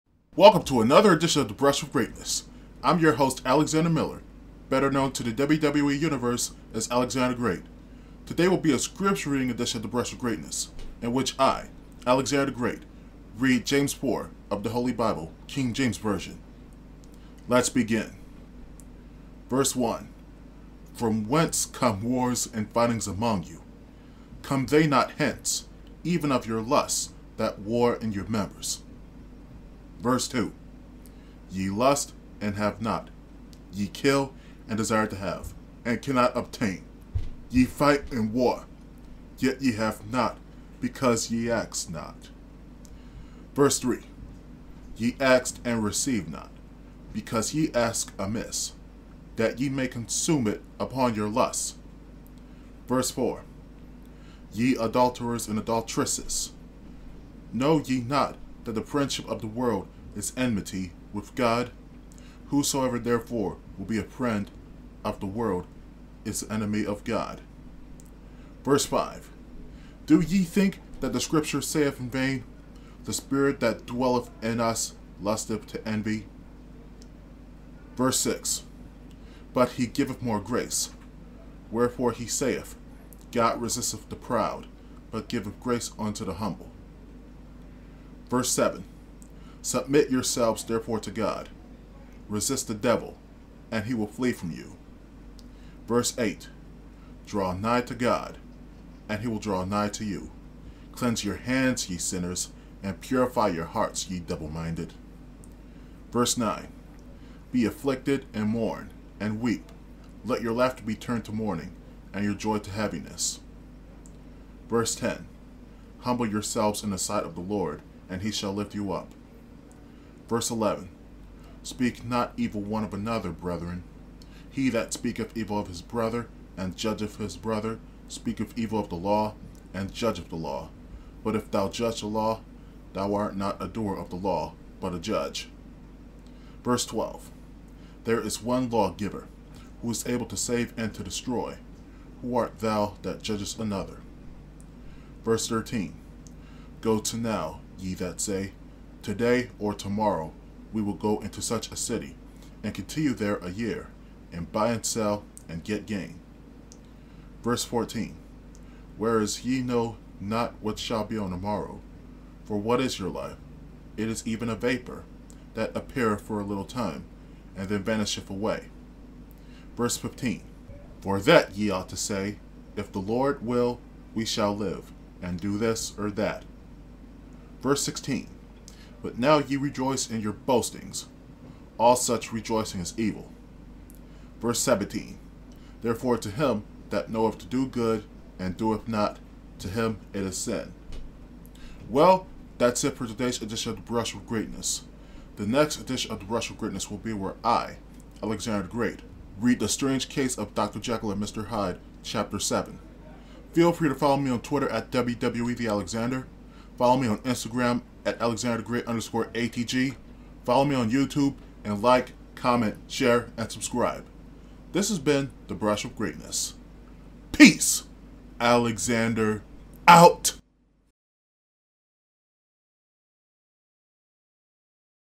The Brush With Greatness Episode 36: Alexander The Great's Reading of James 4
Welcome to another edition of The Brush With Greatness! On today's edition, Alexander The Great reads James 4, King James Version!